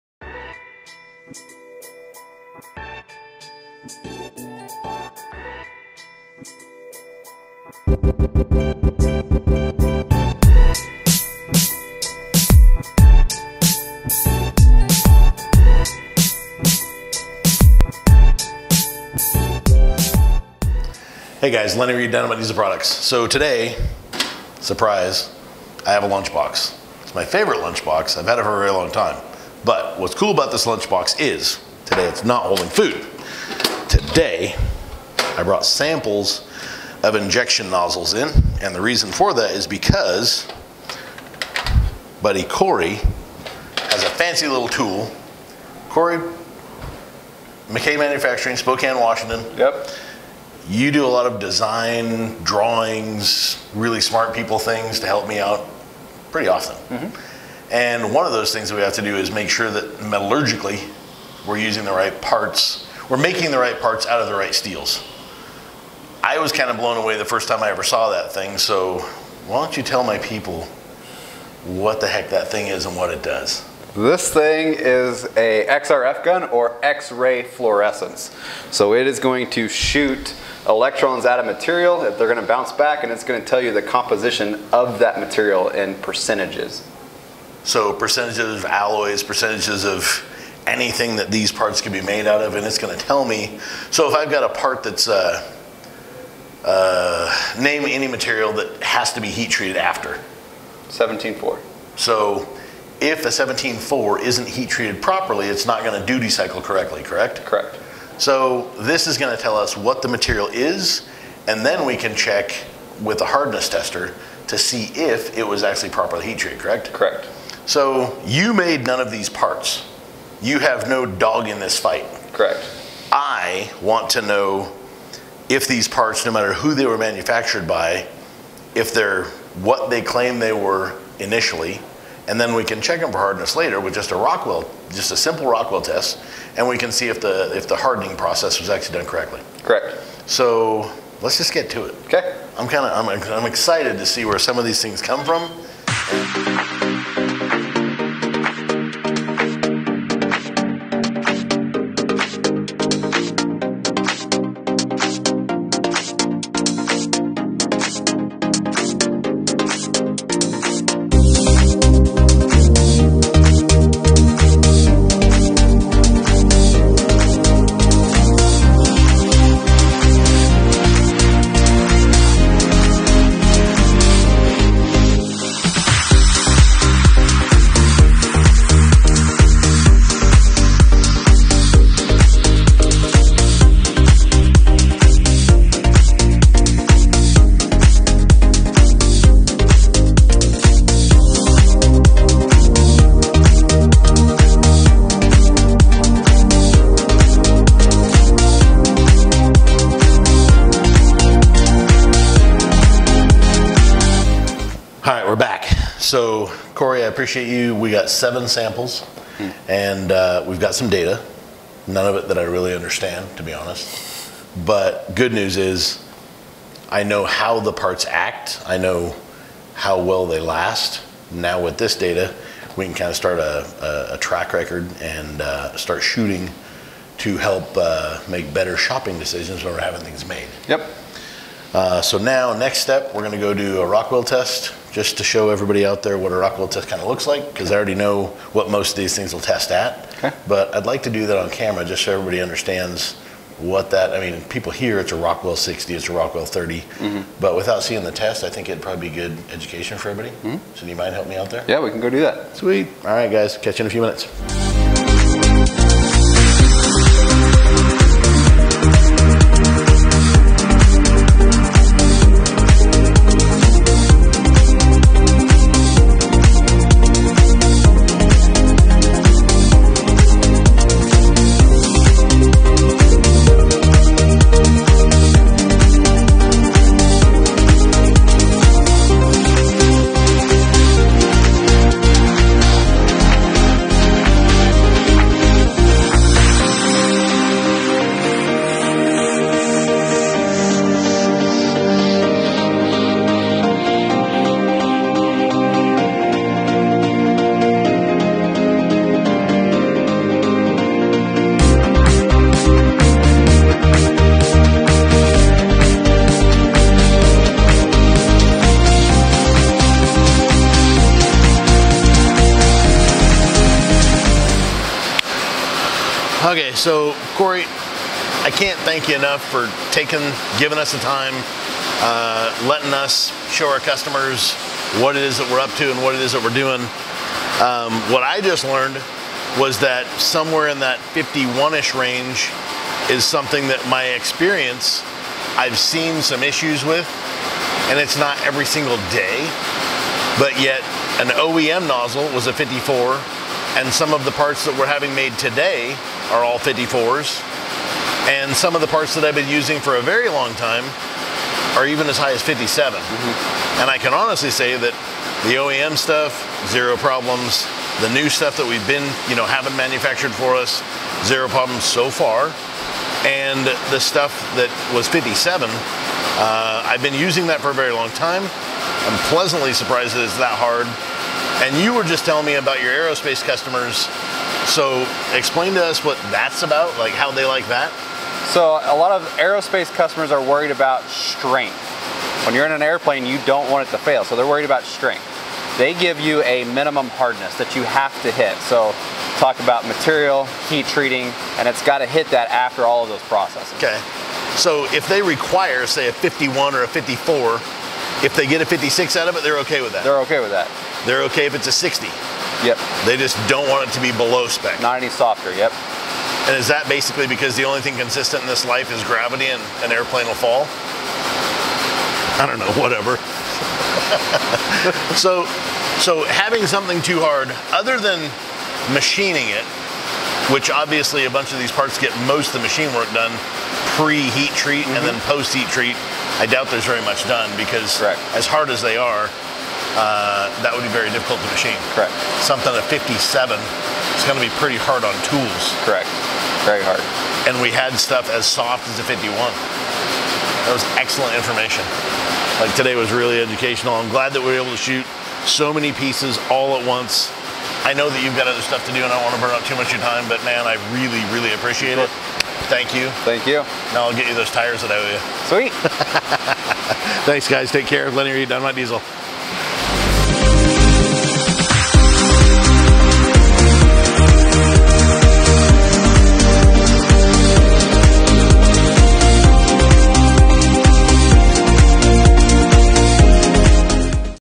Nozzle Composition and Hardness
It's no surprise that we've tested a lot of different nozzle types, and brands, over the years. Here's some of what we've learned about our new blank nozzles!

Our new blank nozzle composition is on par with a Factory Bosch product, and even tested identical on the Rockwell Gauge, with a Rockwell Hardness of 54!